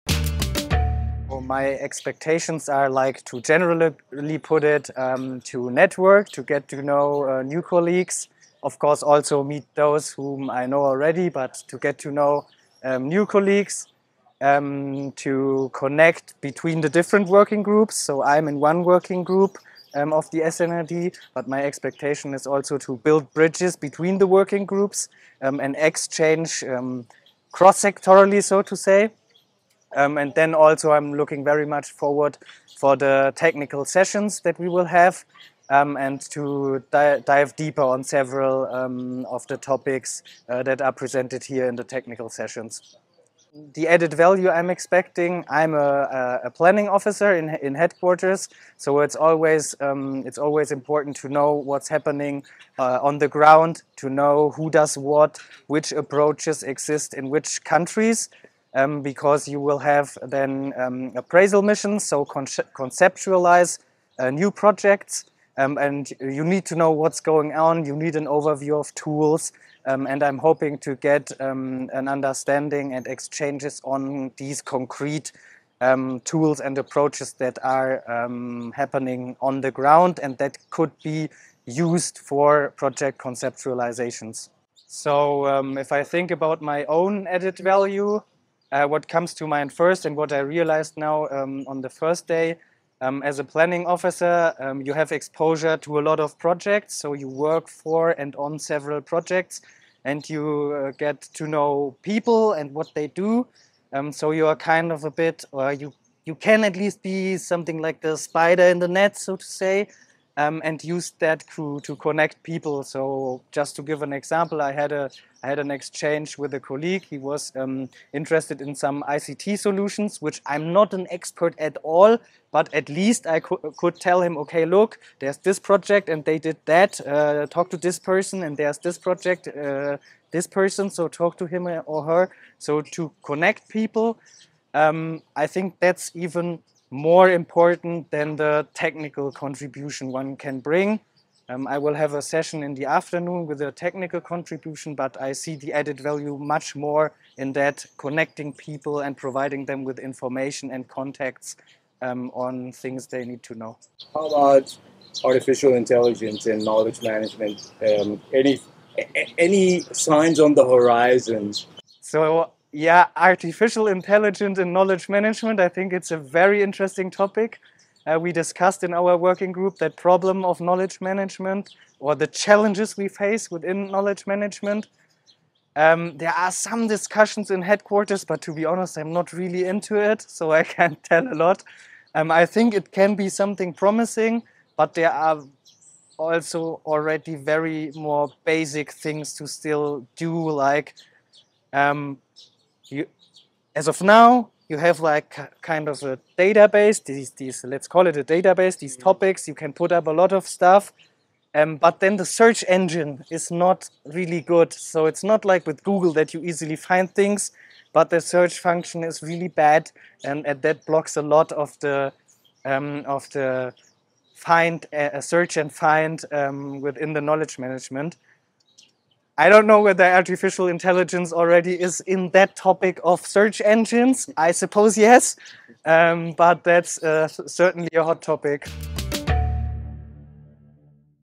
Artificial Intelligence — Certainly a Hot Topic in Knowledge Management
✅ Brief statement by Johannes Peters at the 2023 SNRD Africa conference "Transform & Innovate: Future-proof Food Systems", which was held in Somone/Senegal, 8-13 October.

Hannes works in GIZ's Sectoral Department and accompanies working groups.

He's a tandem partner of SNRD's inclusive agribusiness working group.

SNRD Africa is short for GIZ's Sector Network Rural Development Africa.